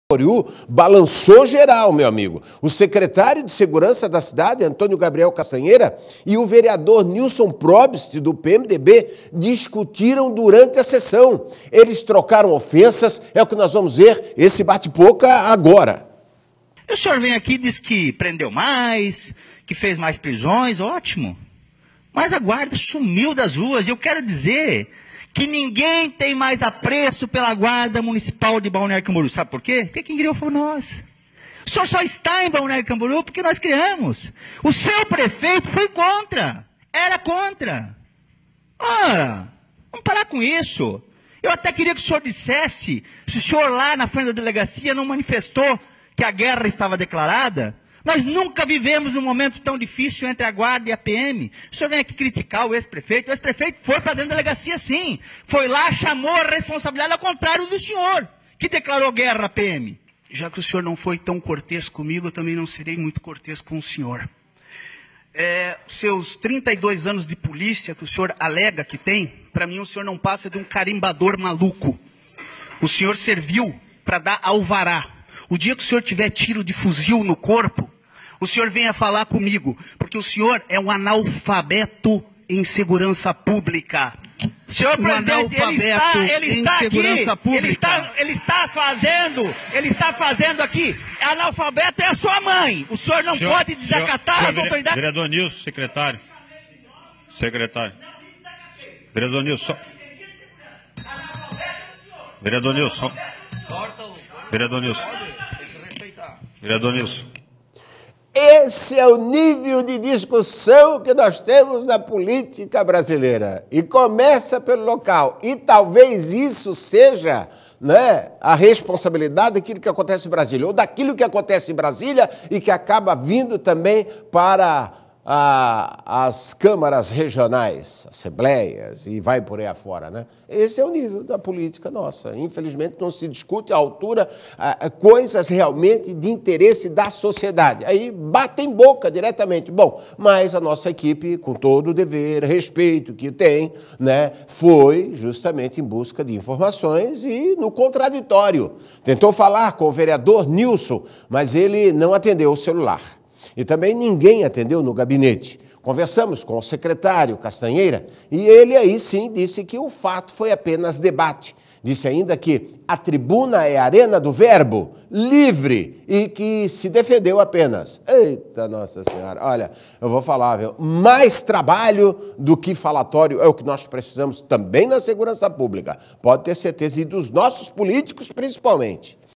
Veja a confusão na Câmara de Vereadores de Balneário Camboriú
Na tarde desta terça-feira (05) teve confusão na sessão da Câmara de Vereadores de Balneário Camboriú. O secretário de segurança da cidade, Antônio Gabriel Castanheira, e o vereador Nilson Probst, do PMDB, discutiram durante a sessão.